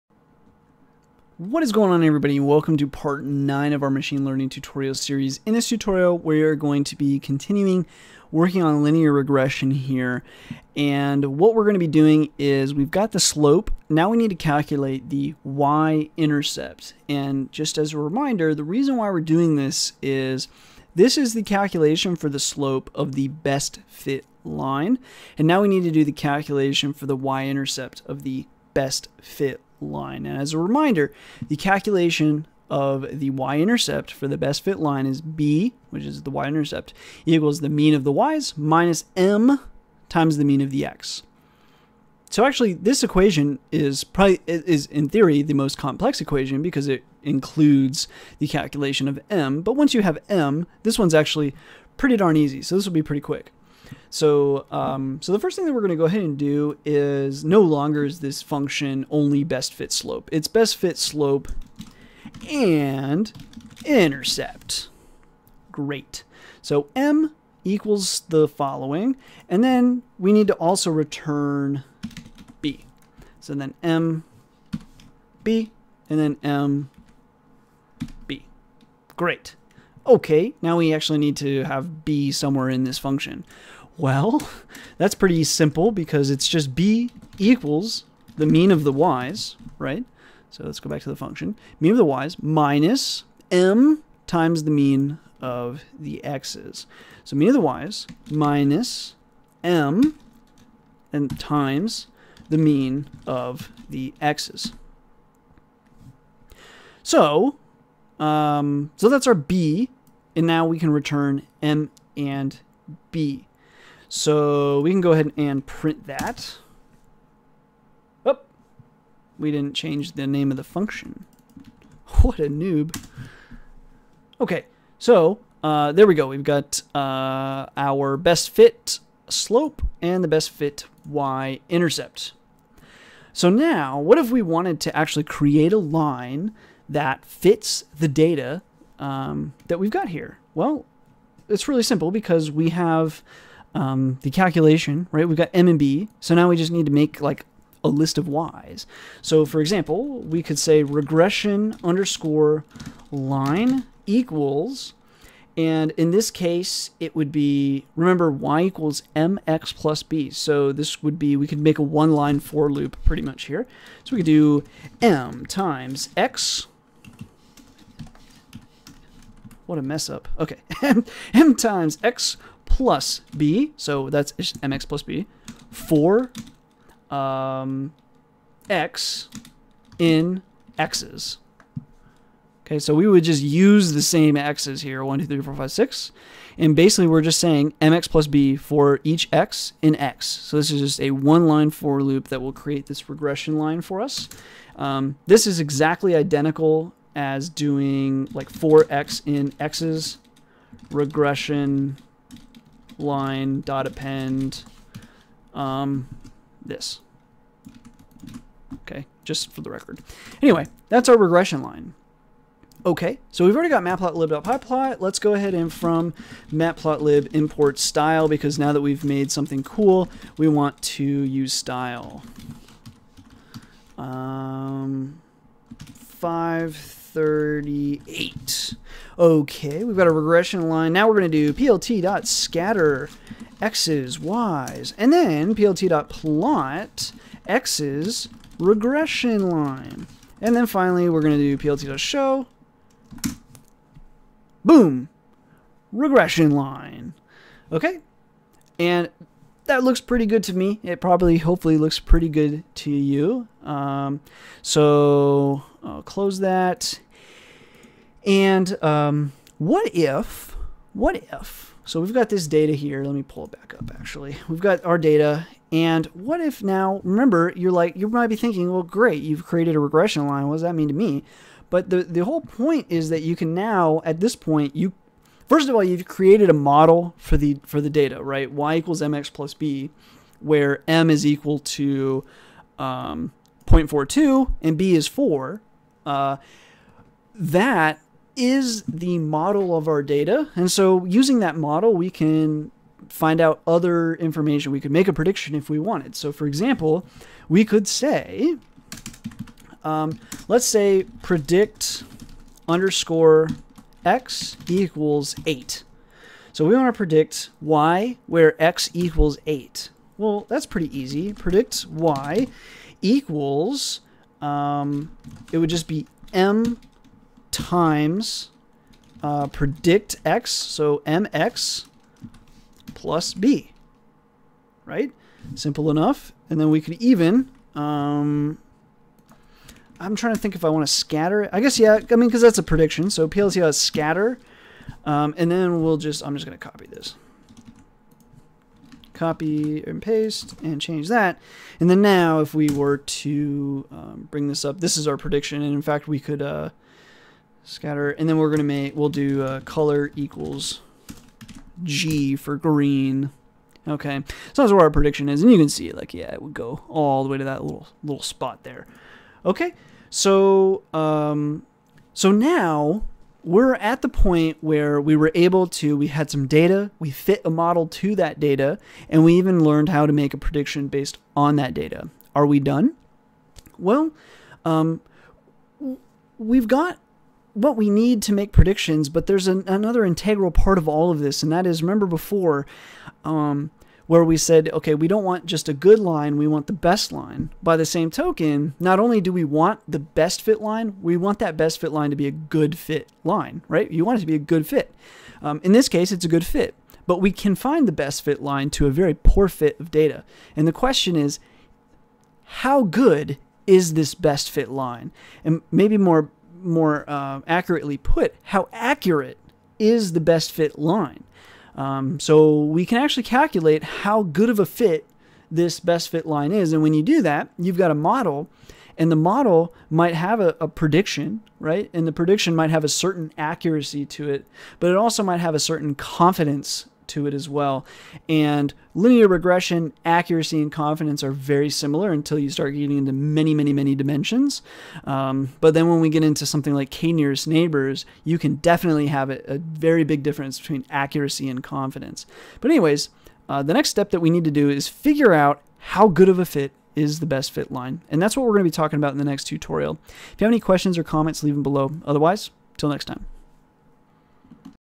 How to program the Best Fit Line - Practical Machine Learning Tutorial with Python p.9
Welcome to the 9th part of our machine learning regression tutorial within our Machine Learning with Python tutorial series. We've been working on calculating the regression, or best-fit, line for a given dataset in Python. Previously, we wrote a function that will gather the slope, and now we need to calculate the y-intercept.